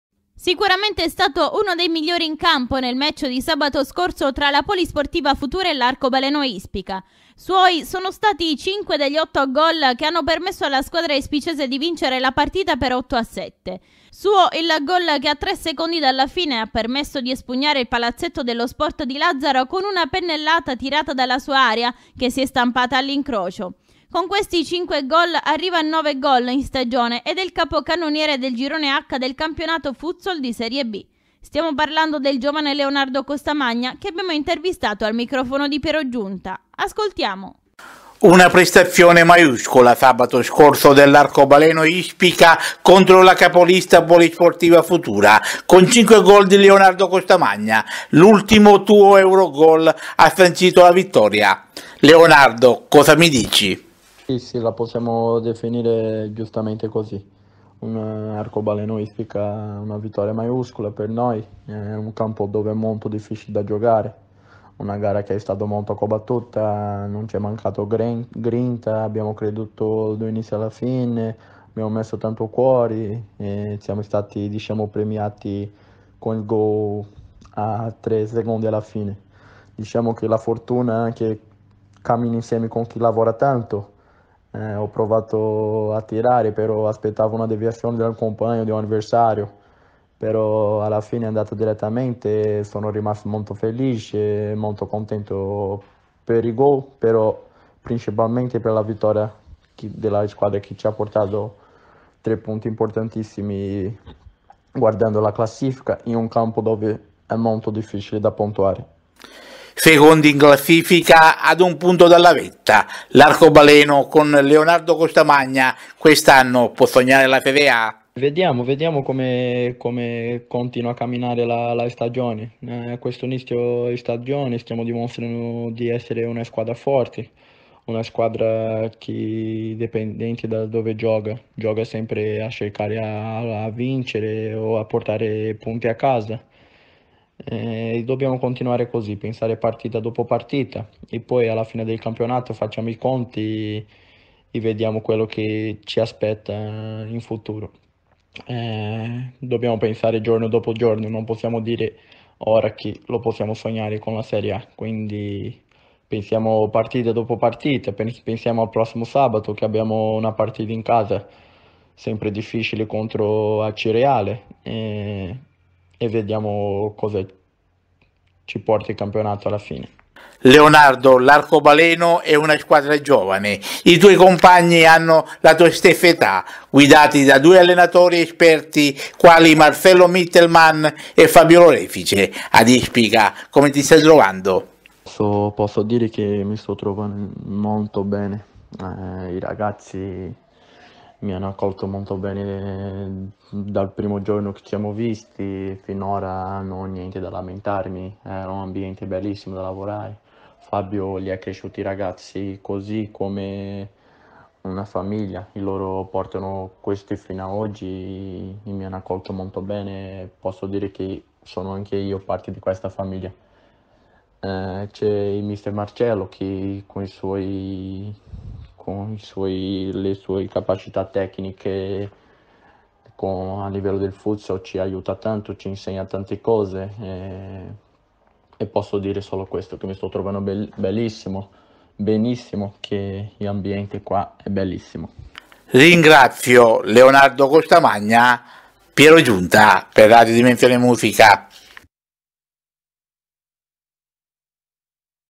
QUATTRO CHIACCHIERE RADIOFONICHE - INTERVISTA A LEONARDO COSTAMANHA
Quattro chiacchiere ed un caffè con: L'Arcobaleno Ispica
Squadra ispicese militante nel campionato Futsal di serie B

Sicuramente è stato uno dei migliori in campo nel match di sabato scorso tra la Polisportiva Futura e l’Arcobaleno Ispica. Suoi sono stati cinque degli otto gol che hanno permesso alla squadra ispicese di vincere la partita per 8 a 7. Suo il gol che a tre secondi dalla fine ha permesso di espugnare il Palazzetto dello sport di Lazzaro con una pennellata tirata dalla sua area che si è stampata all’incrocio. Con questi 5 gol arriva a 9 gol in stagione ed è il capocannoniere del girone H del campionato Futsal di serie B. Stiamo parlando del giovane Leonardo Costamanha che abbiamo intervistato al microfono di Piero Giunta. Ascoltiamo.